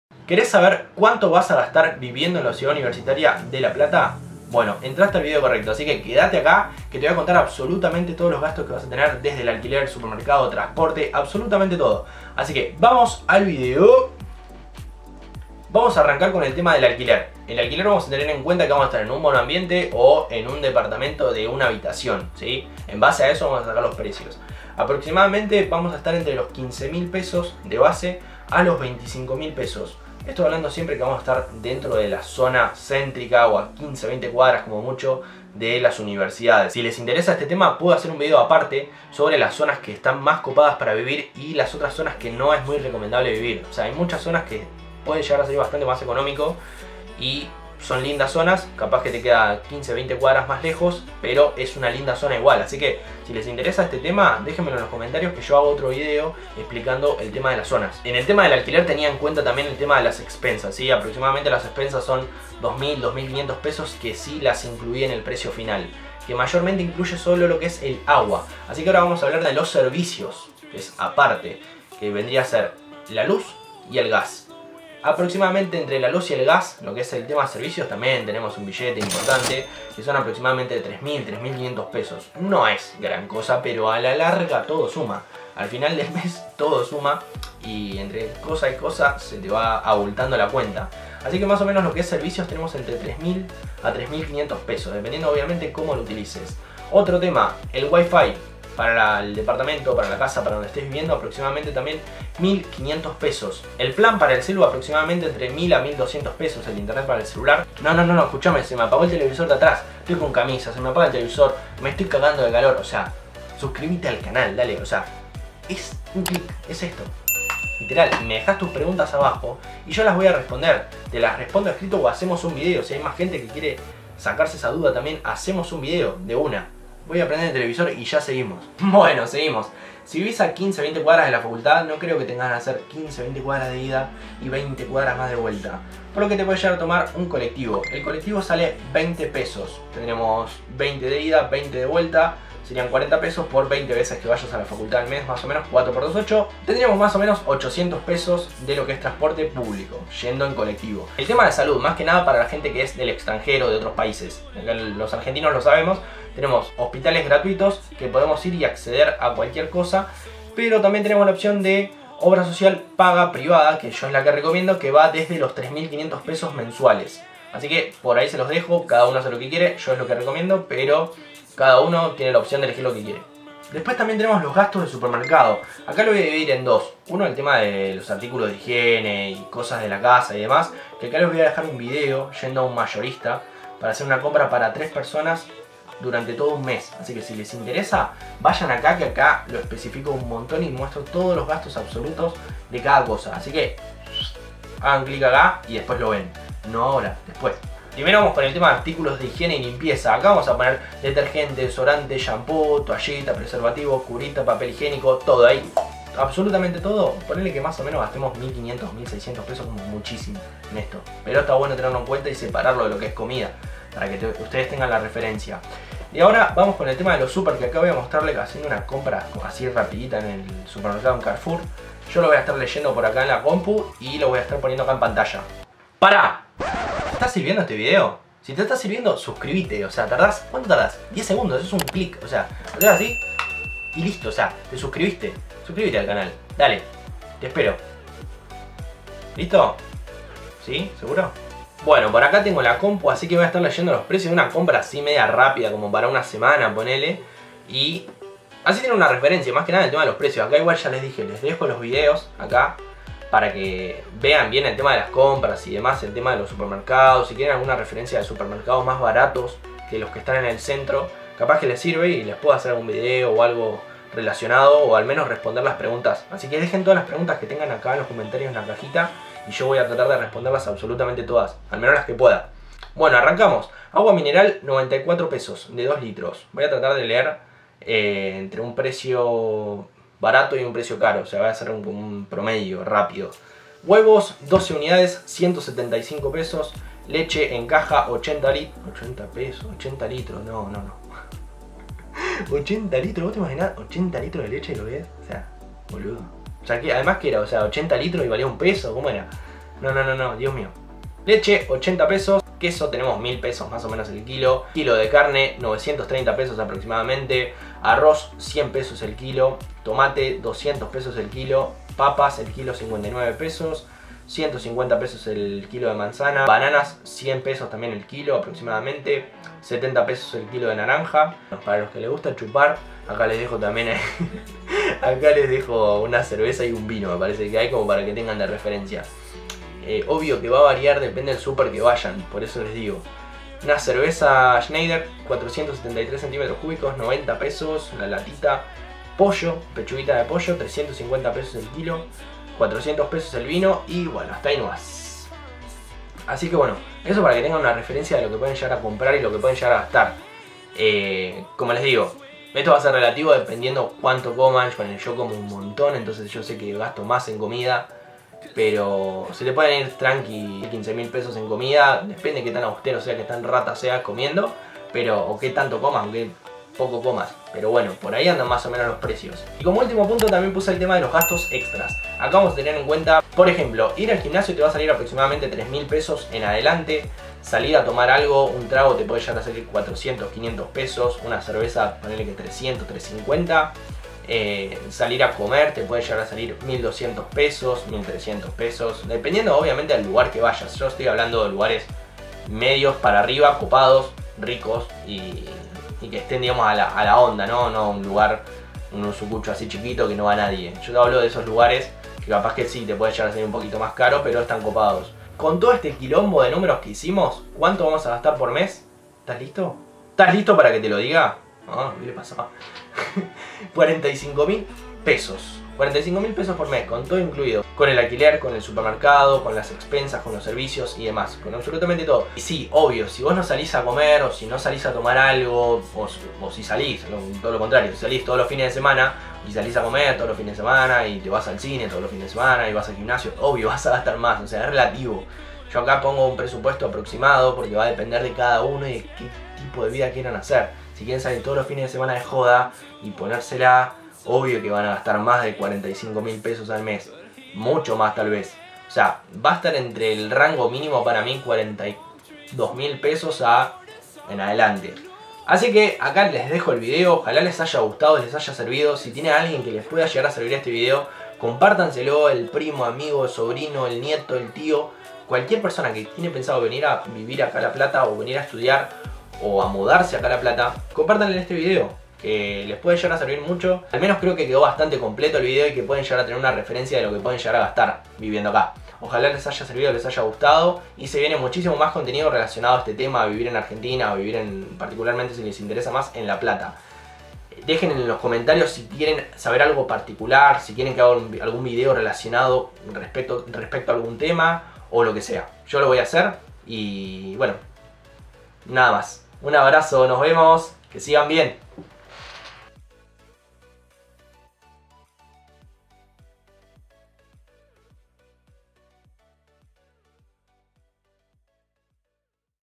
¿CUÁNTO CUESTA VIVIR en la ciudad de LA PLATA 💵? (La verdad)
Hoy les traigo un video sobre el costo de vida en La Plata, la ciudad de las diagonales, la famosa Ciudad Universitaria.
Tocamos puntos como, el alquiler, supermercado, servicios y extras⚡️
Si queres mas contenido de la Ciudad Universitaria de La Plata, déjame un comentario diciendo sobre que tema queres que hable y lo hago en video.

Marcas de tiempo:
00:00 Inicio
00:18 Alquiler
01:19 Servicios
01:38 WiFi y Celular
02:10 Transporte
02:31 Salud
02:54 Supermercado
03:45 PARÁ!
06:10 Extras
06:48 Gasto total